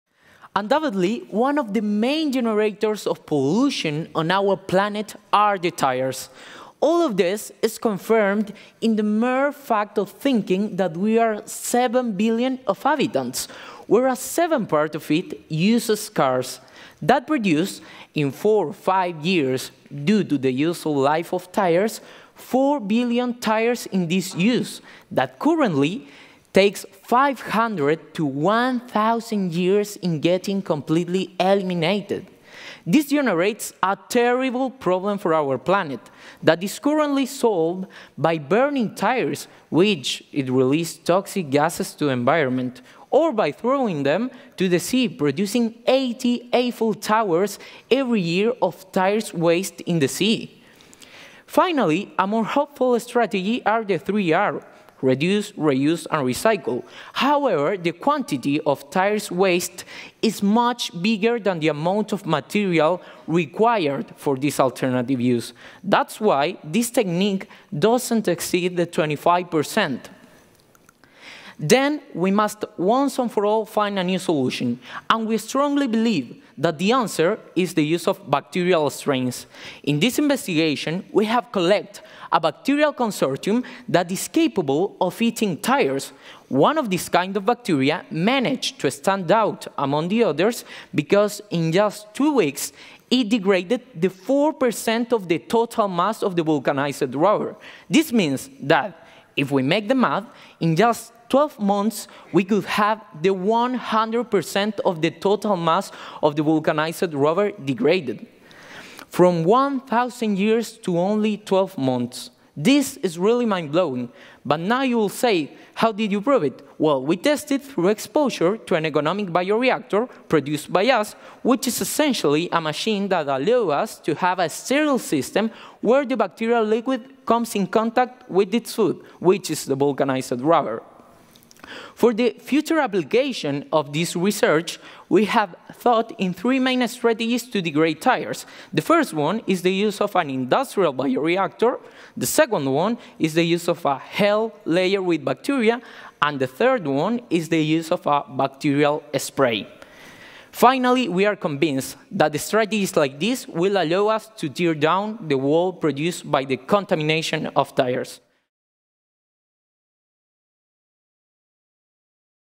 Falling Walls Lab 2019 – Jorge Miles: Breaking The Wall of Tyres Pollution
Jorge Miles, Chile